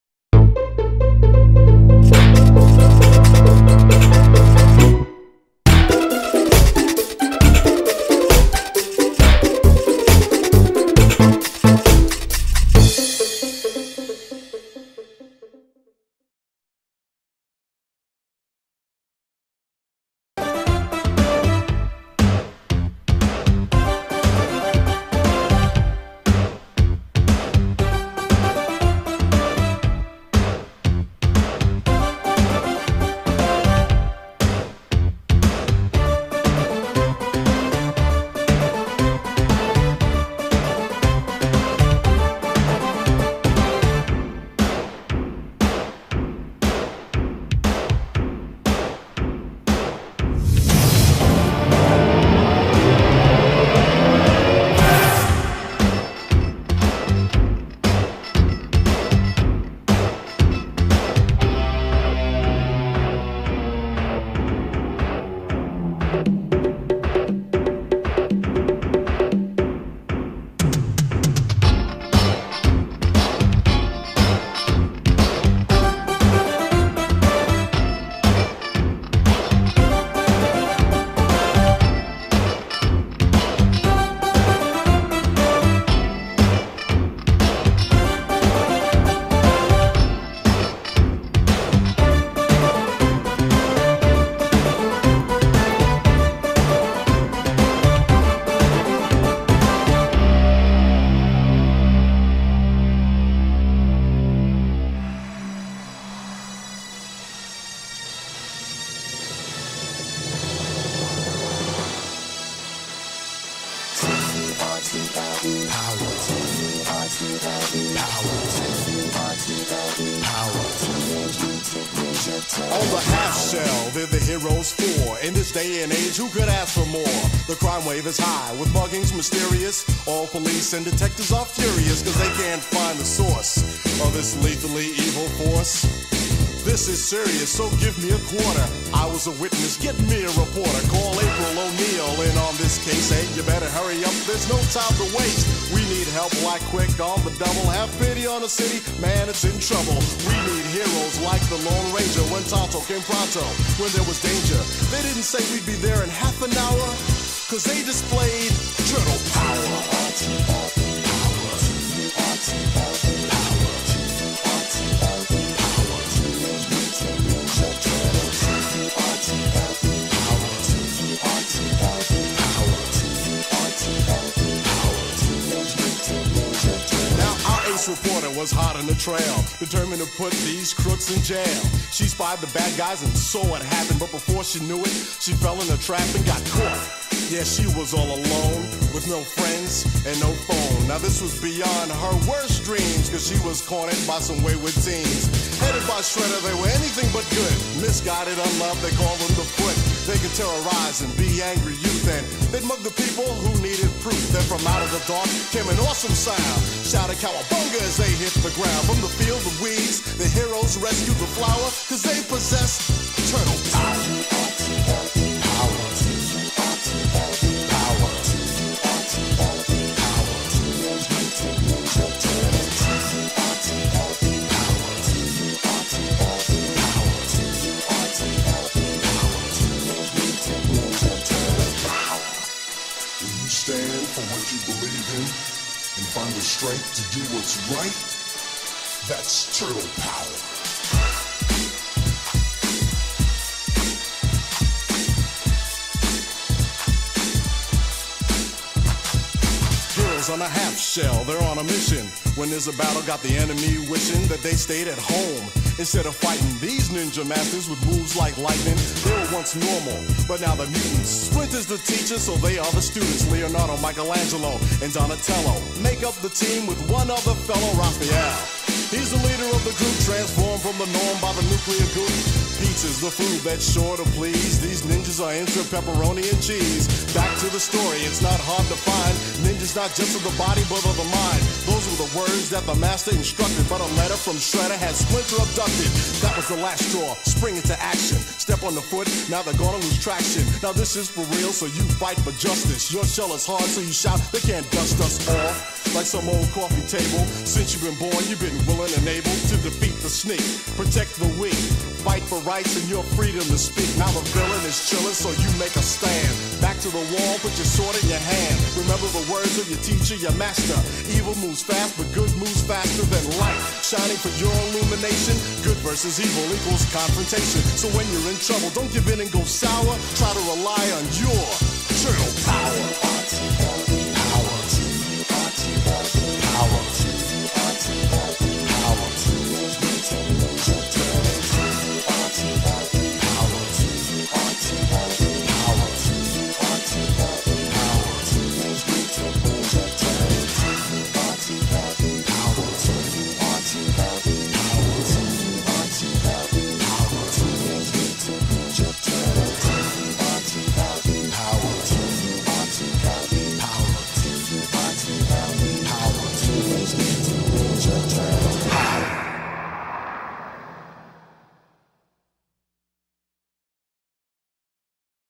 Teenage Mutant Ninja Turtles: Creating a Bookcase Diorama
NECA Toys have done wonders on recreating those Heroes in a half shell in action figure form!

Another franchise I had loved as a kid and still do to this day, here is another diorama video, detailing the steps made to produce something worthy to showcase my turtles!

Cowabunga!!